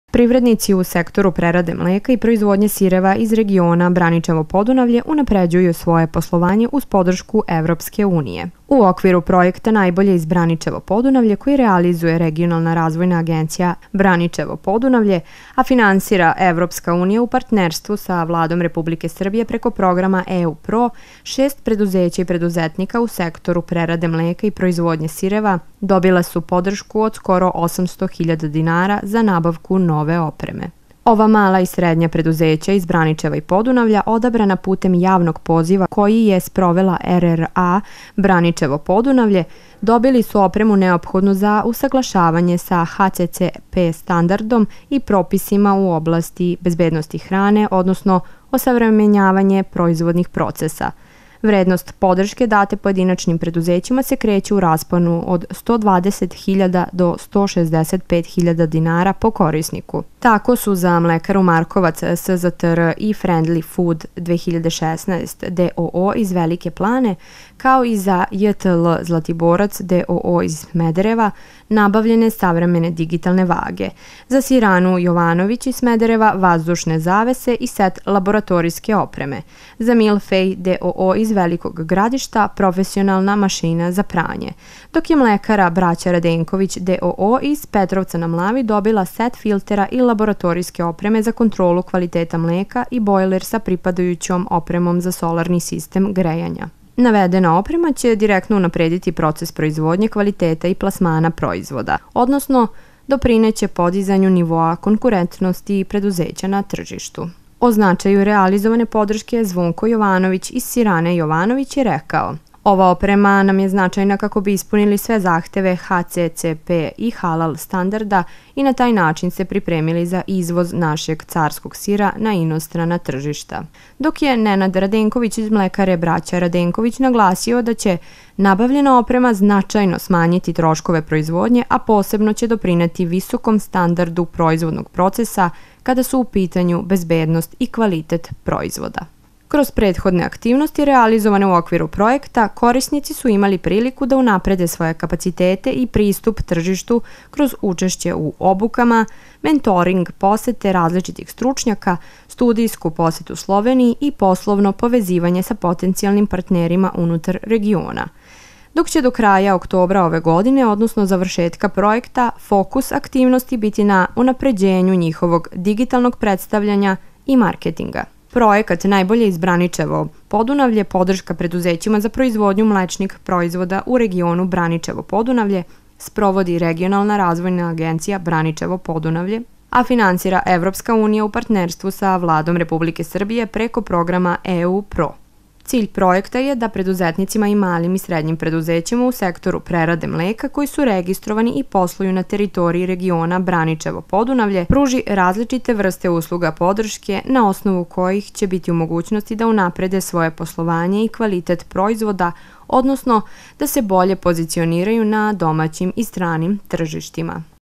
Privrednici iz regiona Braničevo-Podunavlje unapređuju poslovanje uz podršku EU (RTV MLAVA 1.9.20)
Privrednici u sektoru prerade mleka i proizvodnje sireva iz regiona Braničevo-Podunavlje unapređuju svoje poslovanje uz podršku Evropske unije